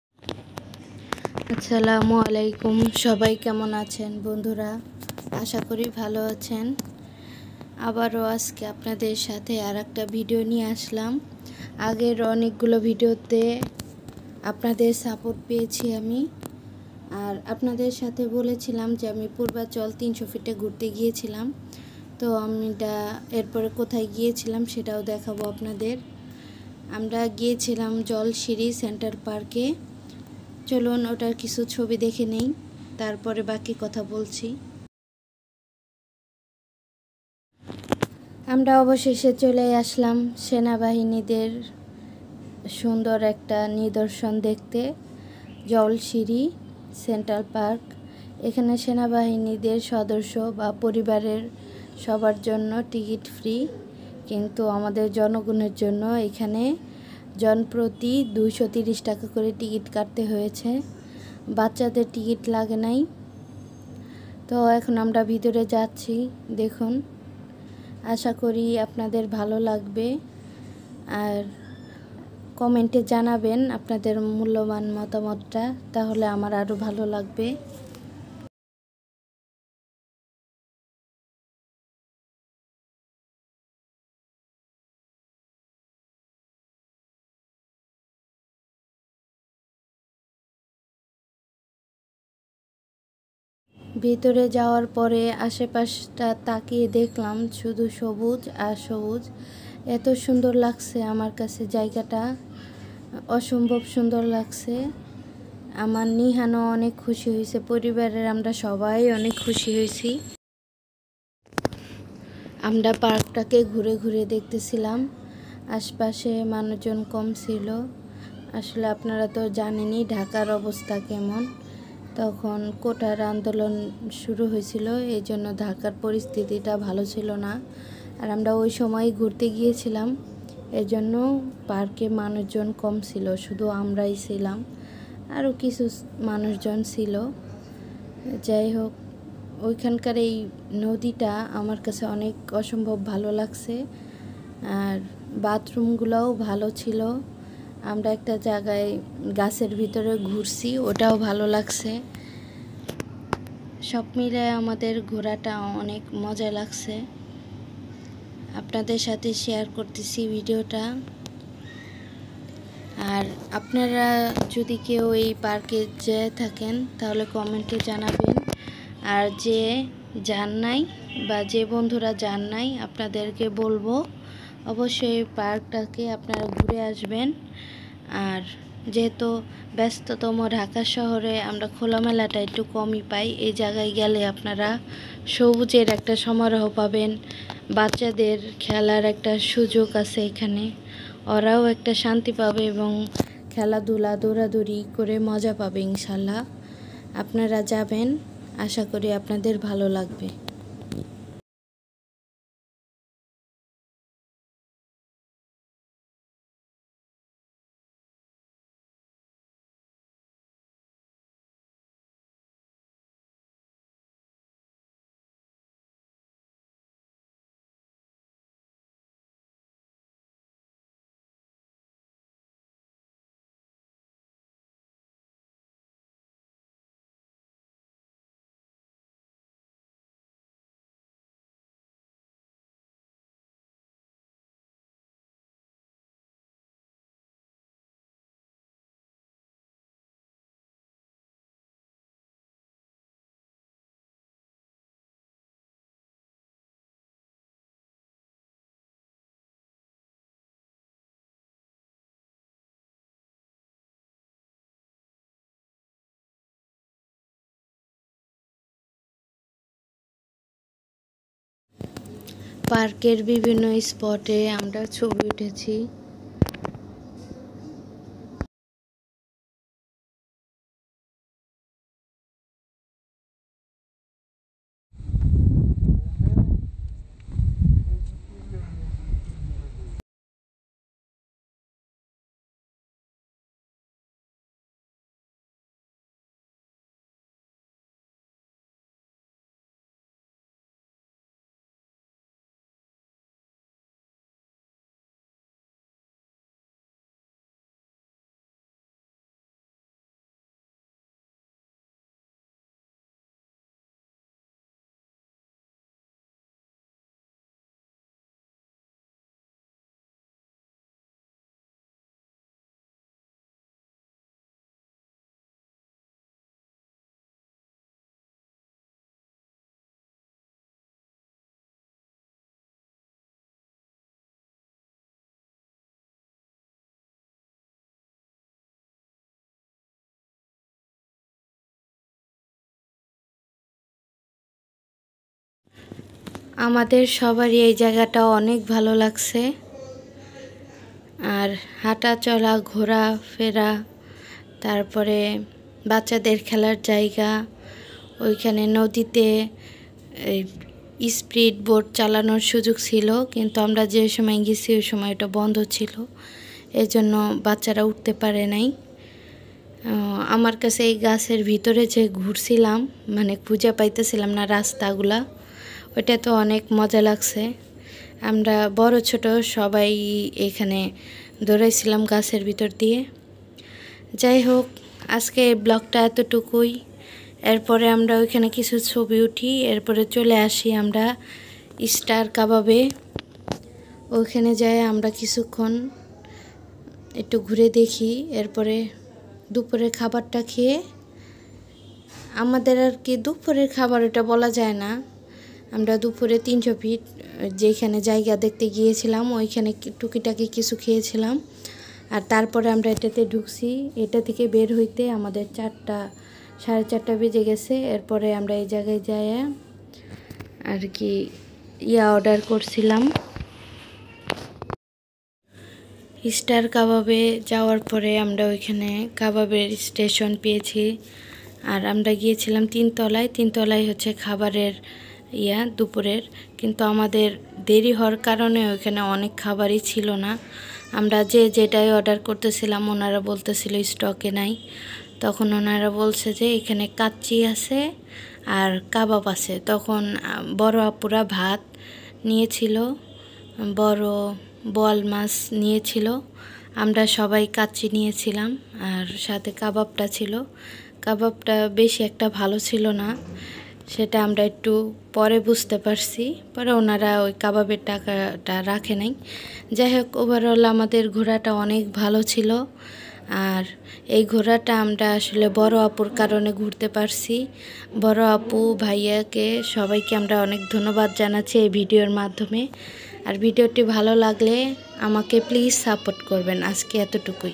আমরা পরিবারের সবাই মিলে (জলসিড়ি সেন্ট্রাল পার্কে) ঘুরতে গিয়েছিলাম।
আশা করি ভিডিওটা আপনাদের ভালো লাগবে।